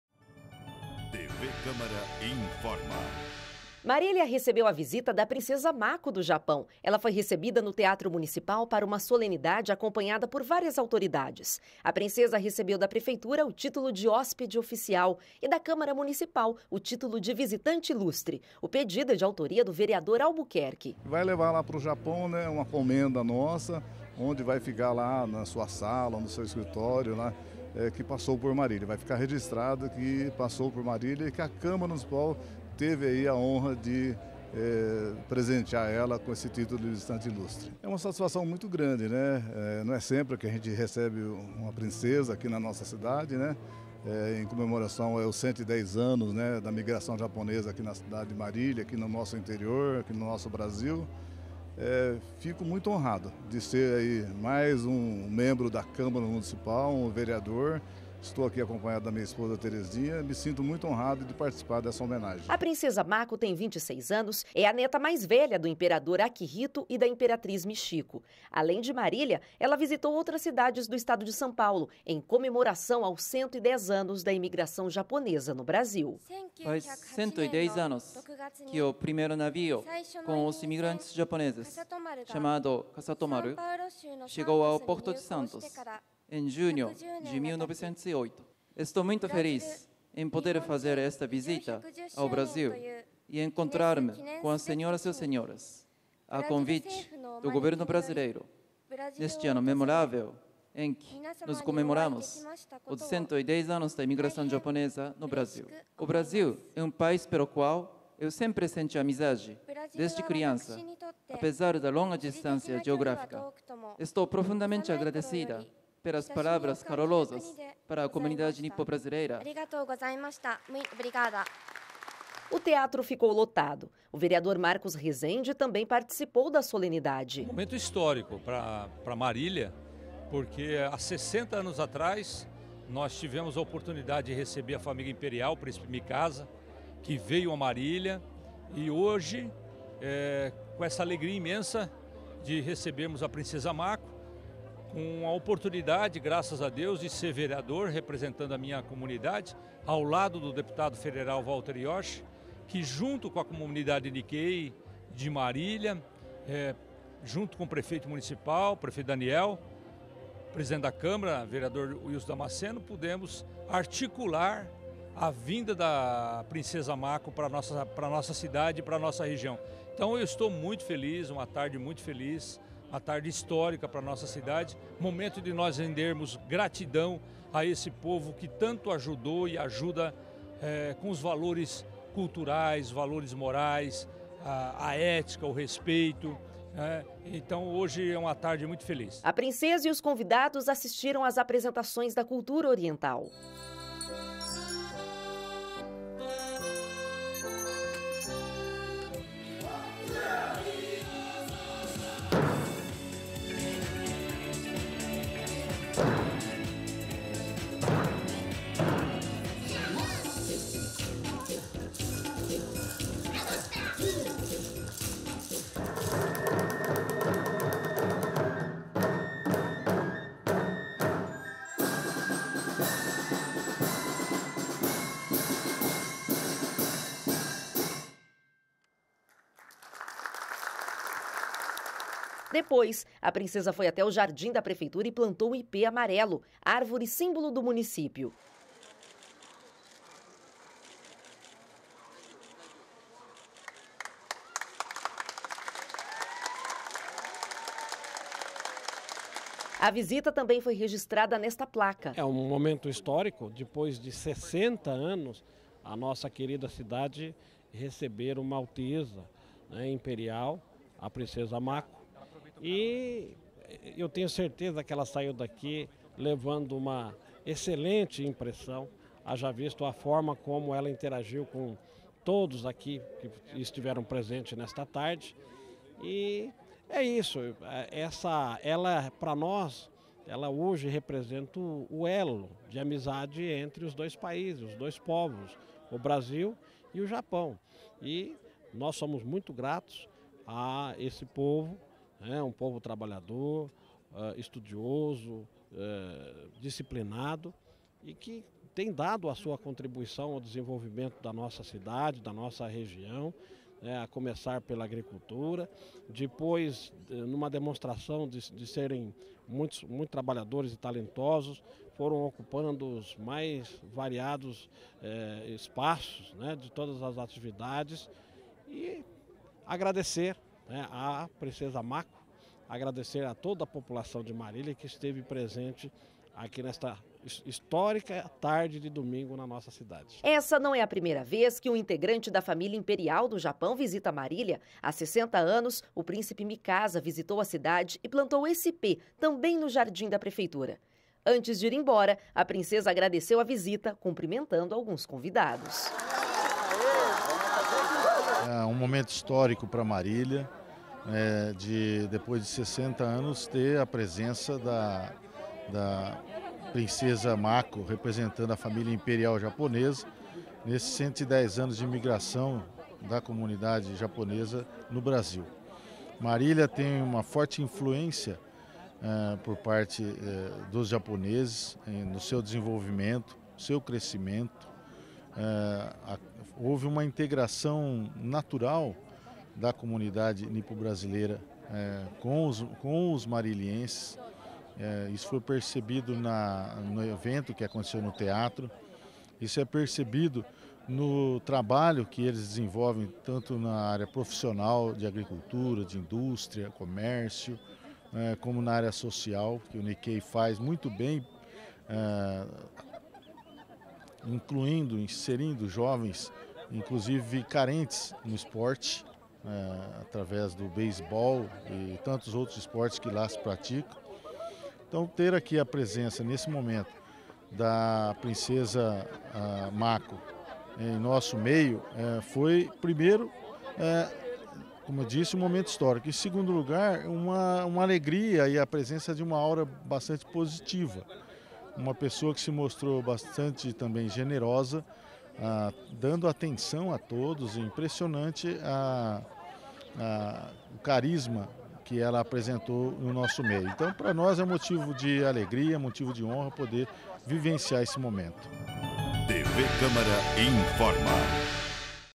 CÂMARA MUNICIPAL ENTREGA TÍTULO DE VISITANTE ILUSTRE A PRINCESA MAKO DO JAPÃO
A Princesa Mako da família imperial do Japão fez uma visita a Marília.
Ela foi recebida no Teatro Municipal para uma solenidade acompanhada por várias autoridades.
A Princesa Mako tem 26 anos e é a neta mais velha do Imperador Akihito e da Imperatriz Michiko.
Além de Marília, ela visitou outras cidades do estado de São Paulo em comemoração aos 110 anos da imigração japonesa no Brasil.
Depois da solenidade, a Princesa plantou um Ipê Amarelo, árvore símbolo do município.